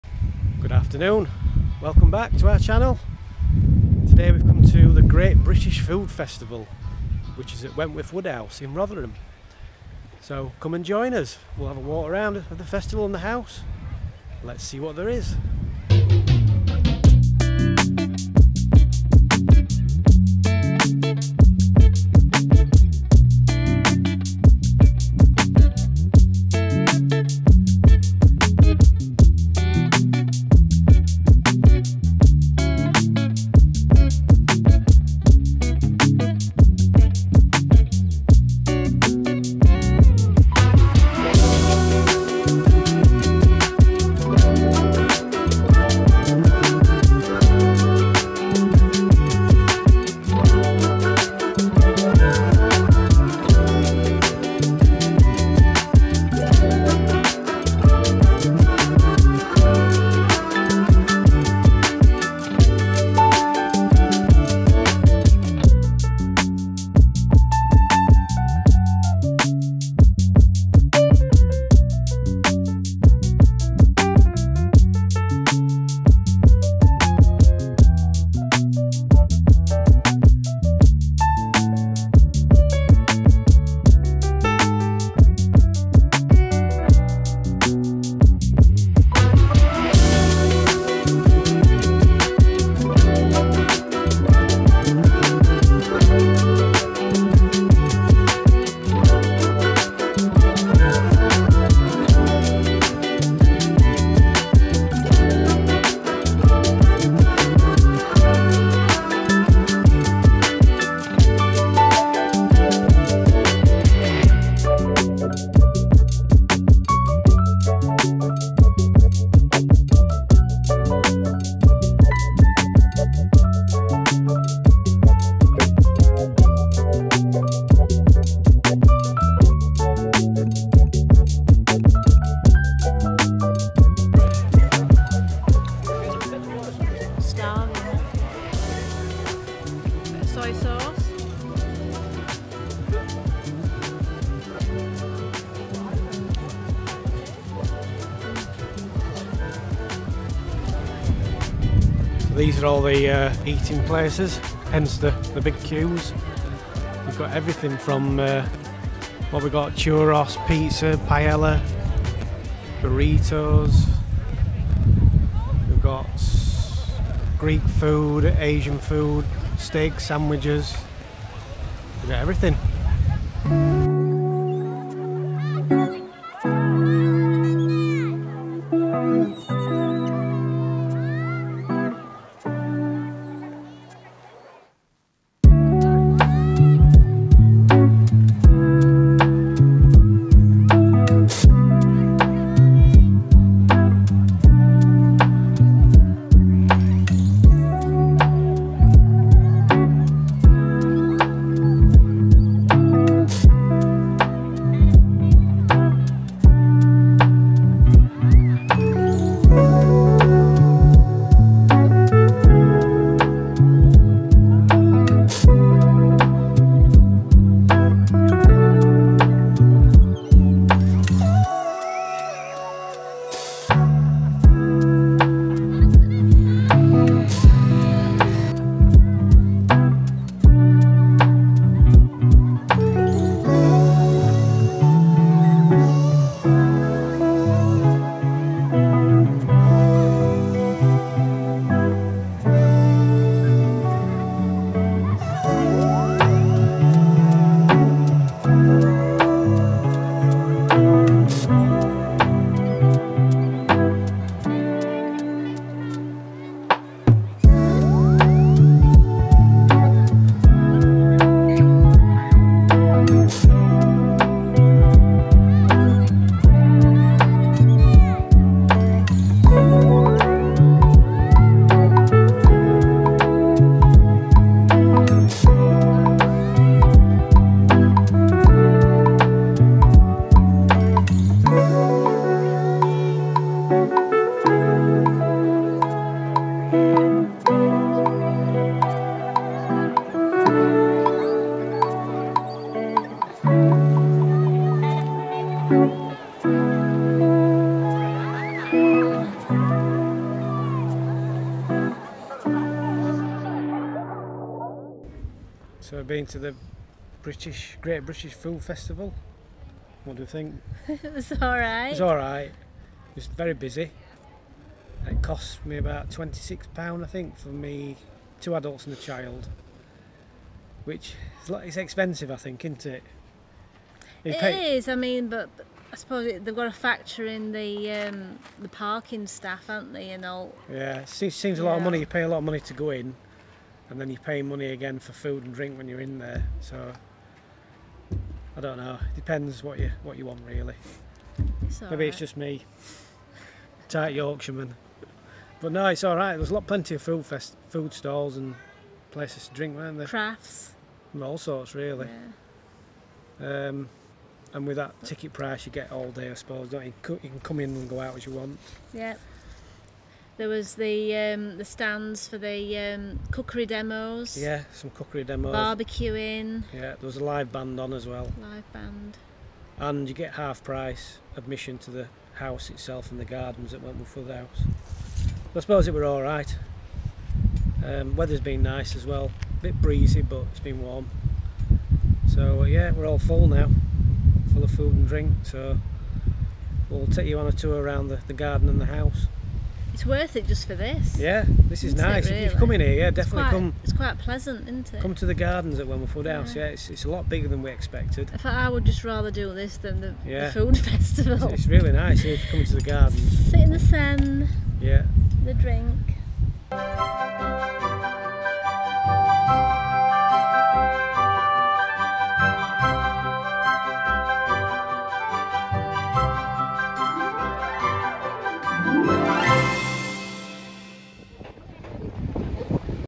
We went to a FOOD FESTIVAL at WENTWORTH WOODHOUSE but was it worth it?
We visited the Great British Food Festival on Saturday 19th August 2003. It was located at the stunning Wentworth Woodhouse in Rotherham, South Yorkshire.

Watch our video to see what we think of this food festival.